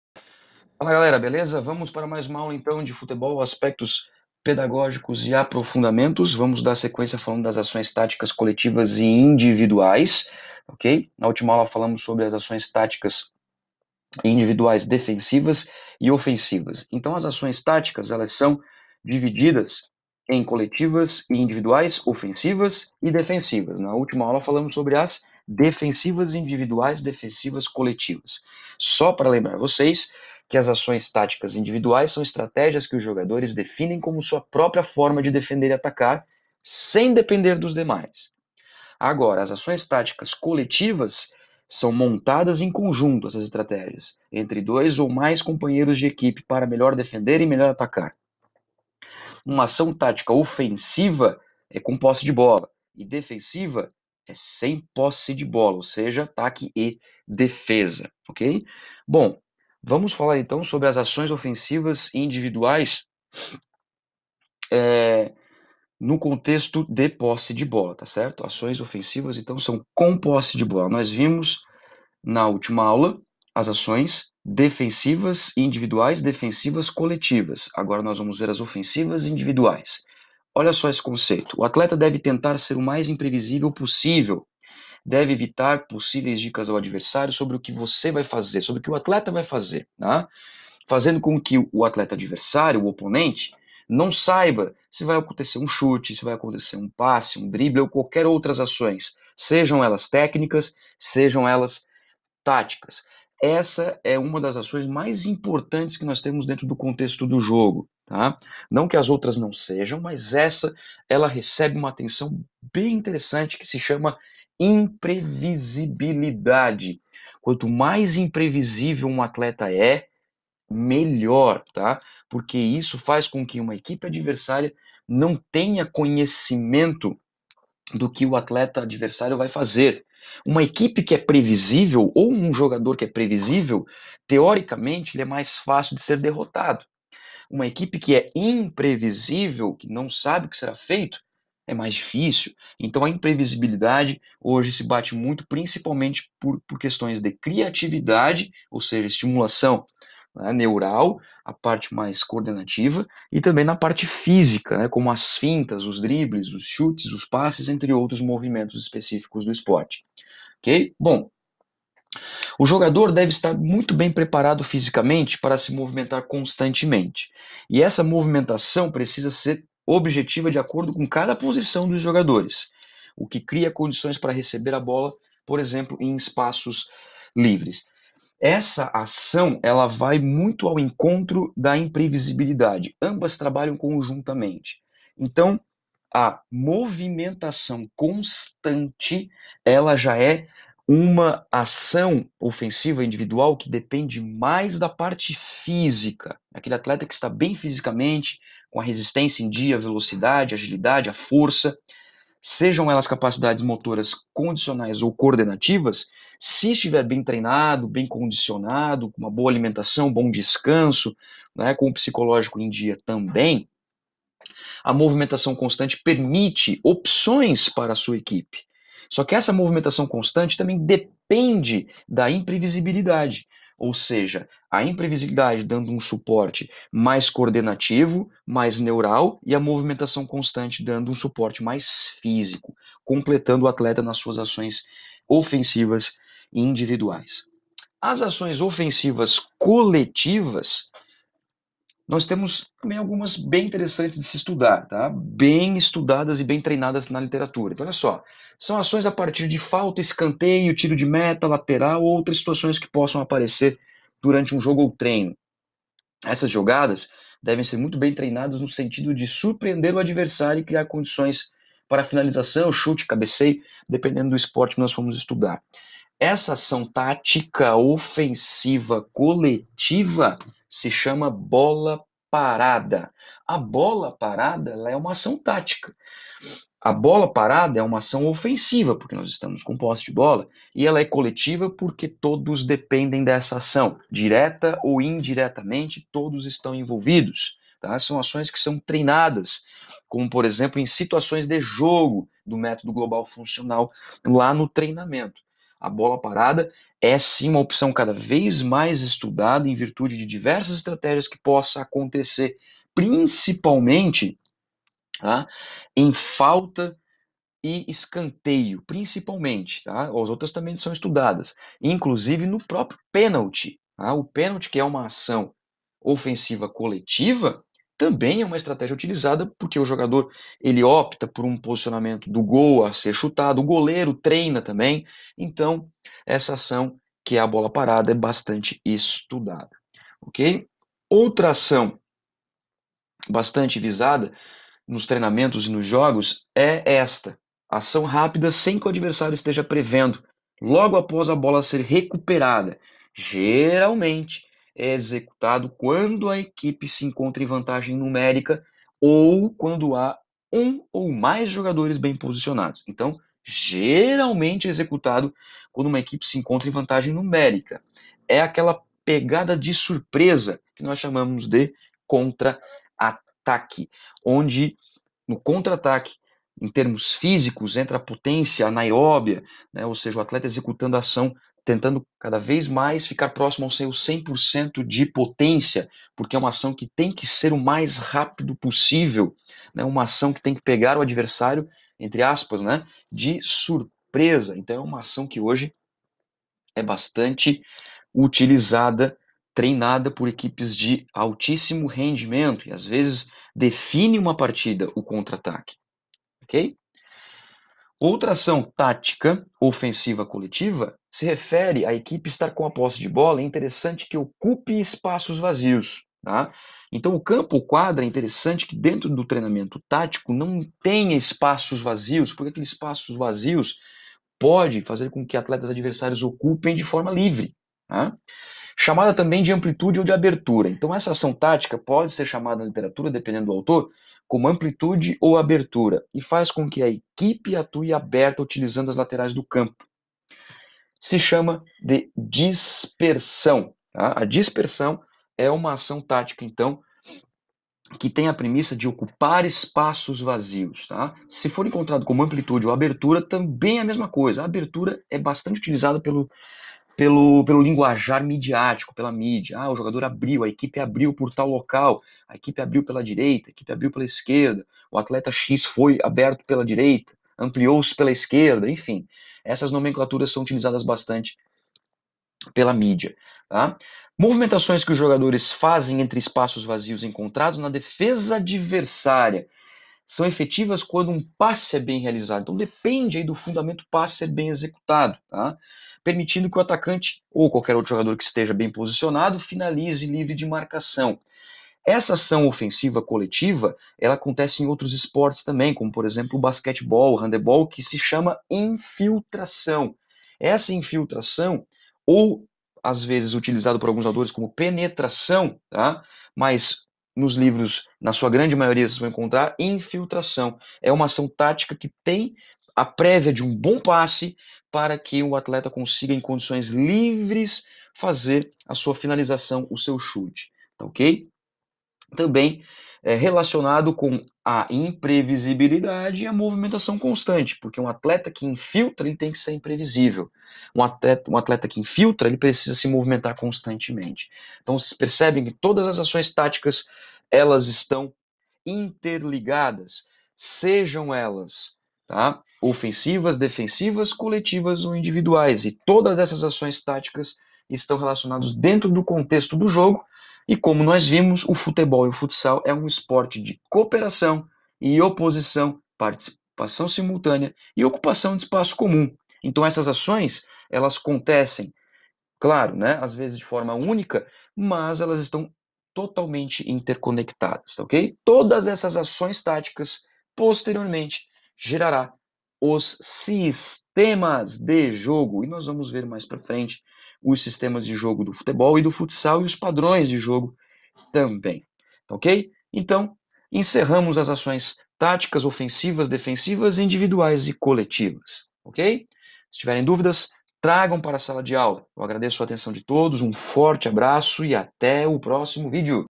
Ações táticas ofensivas individuais e coletivas no Futebol
AULA 10 - O que é uma ação táticas e como elas são divididas

DISCIPLINA: FUTEBOL: ASPECTOS PEDAGÓGICOS E APROFUNDAMENTOS